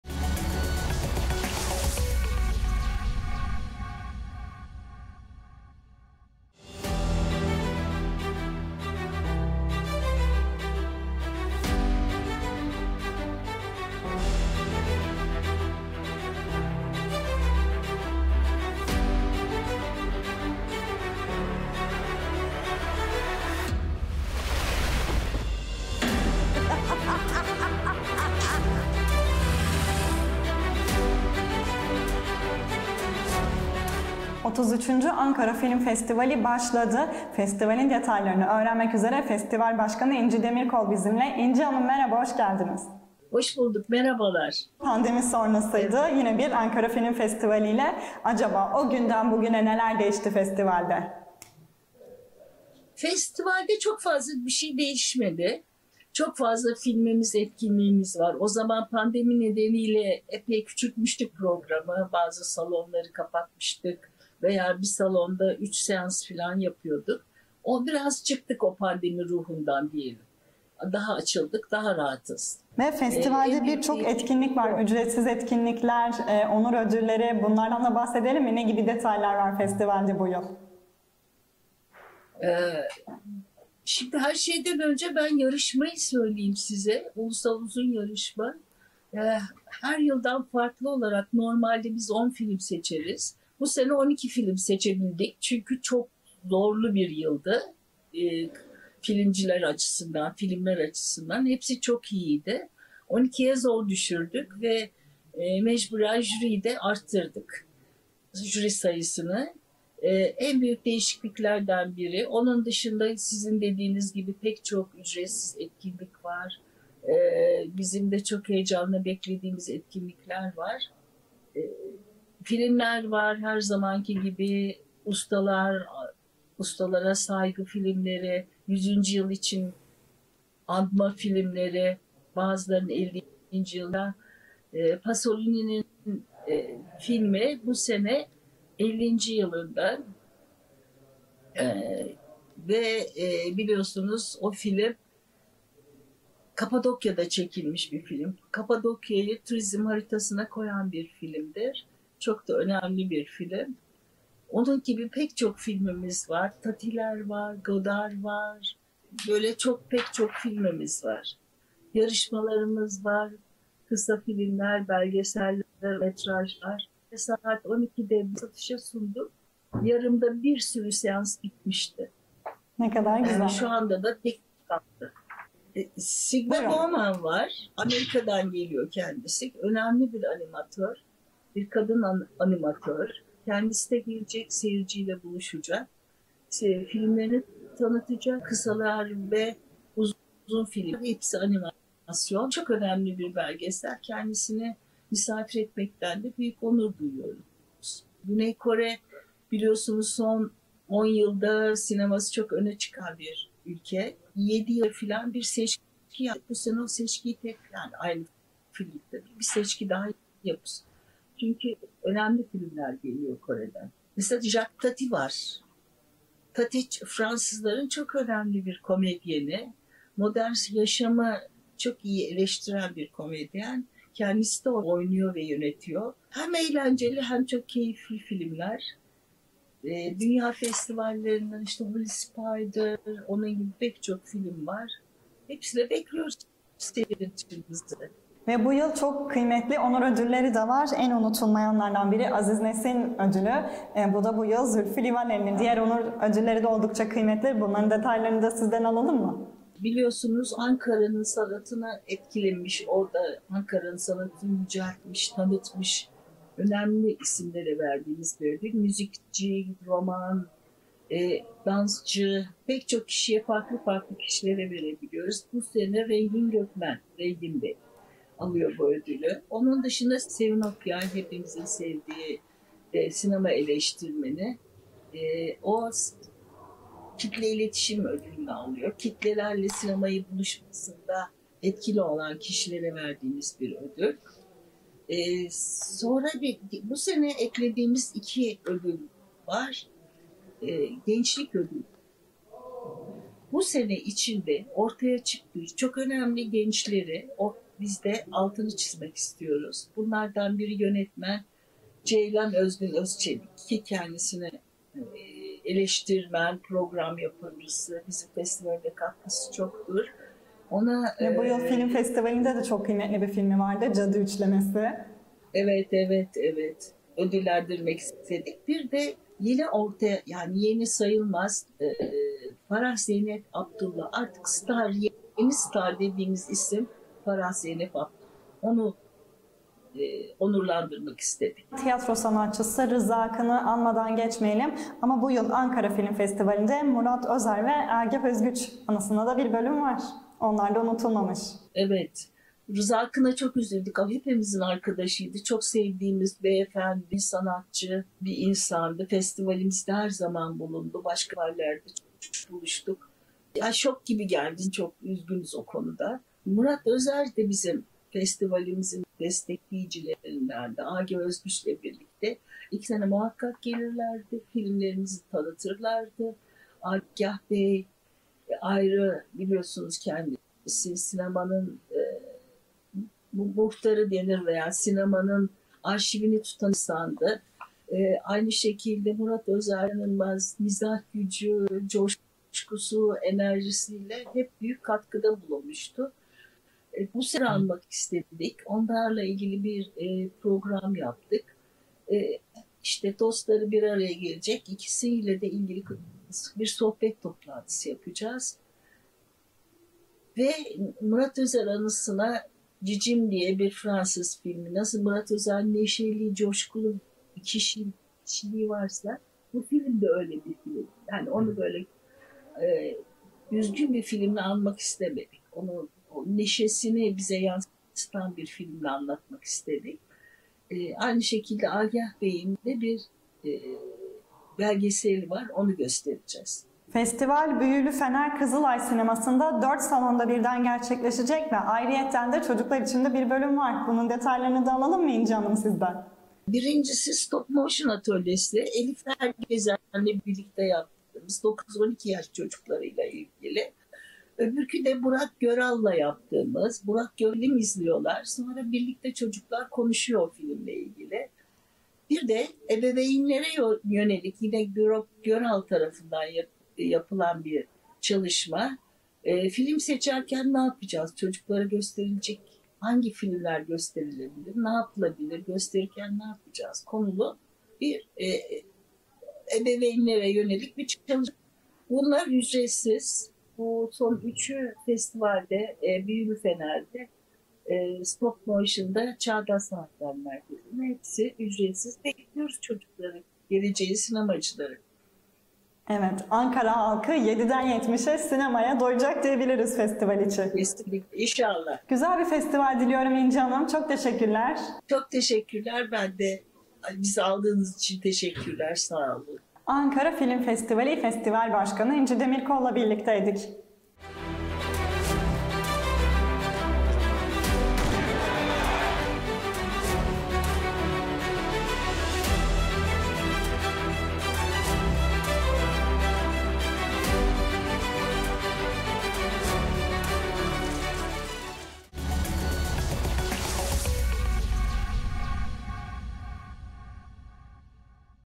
33. Ankara Film Festivali
BU YIL 33. SÜ GERÇEKLEŞEN ANKARA FİLM FESTİVALİ DOPDOLU PROGRAMIYLA BAŞLADI.  7'DEN 70'E HER YAŞA HİTAP EDİCEK FESTİVALİN DETAYLARINI FESTİVAL BAŞKANI İNCİ DEMİRKOL, ECEM KOÇ'A ANLATTI.

Yönetmen - Serpil Boydak
Röportaj & Editör - Ecem Koç
Kameraman – Metin Canım
Montaj – Ecem Kalay